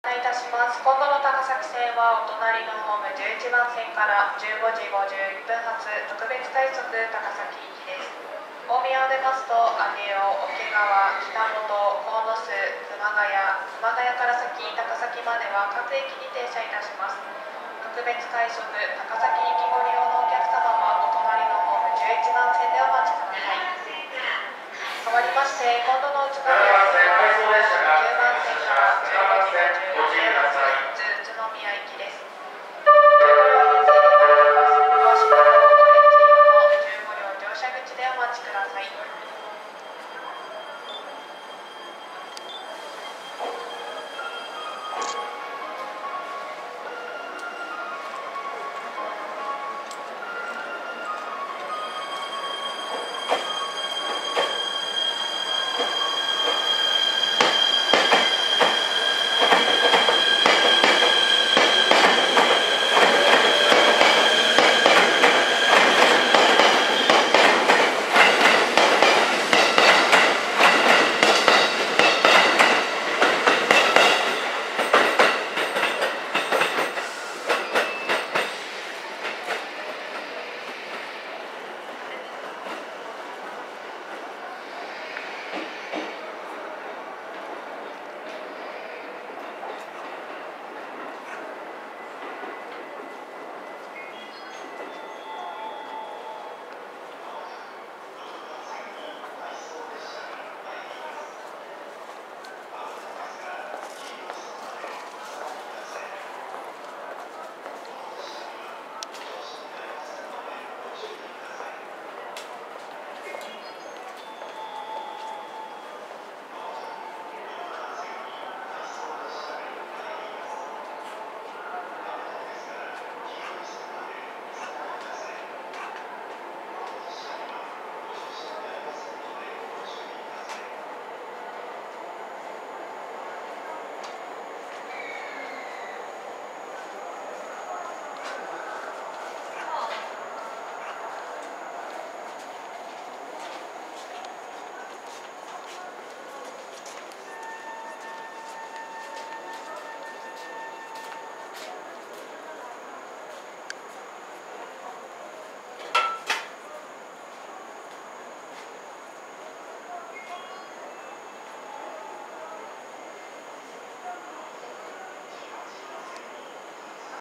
253系1000番台OM‐N01編成 回送電車 大宮発車【ミュージックホーンあり】
2017年2月4日（土）撮影
大宮総合車両センター253系1000番台OM-N01編成（元Ne-201編成）